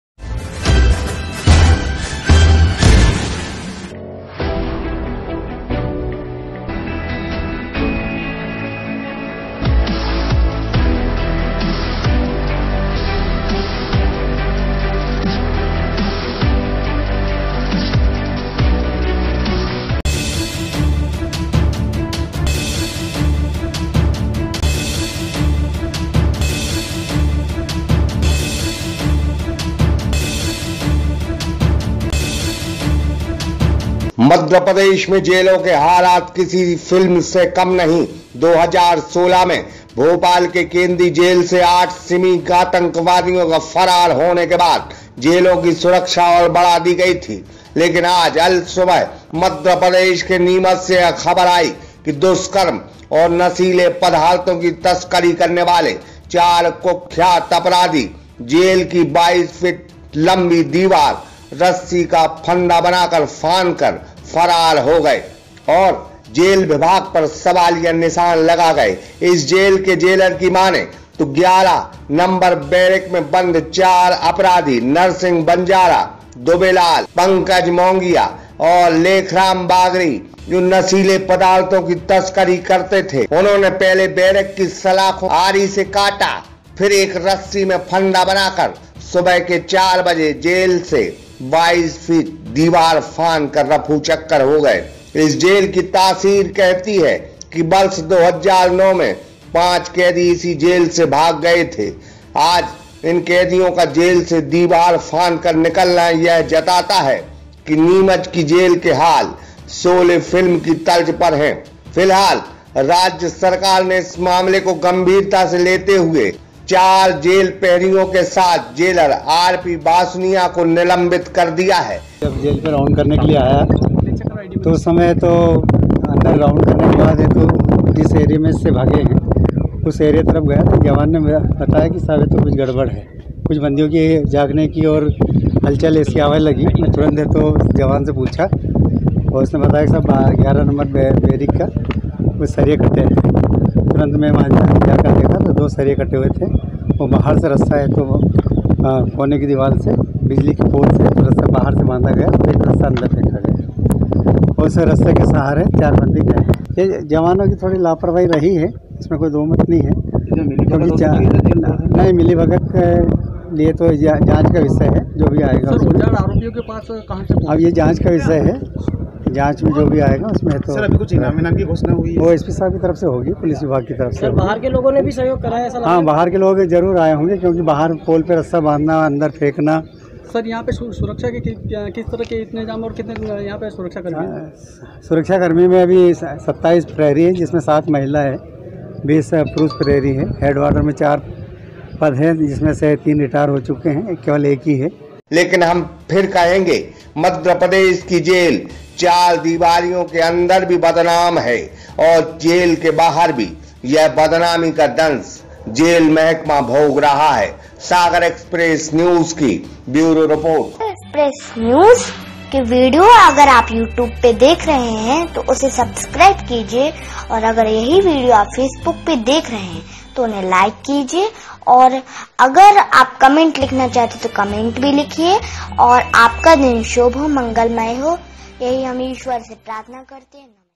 नीमच जेल से फरार इन चार अपराधियों को पहचानो और 50 हजार कमाओ
जेल से भागे आरोपियों पर 50 50 हजार का इनाम जारी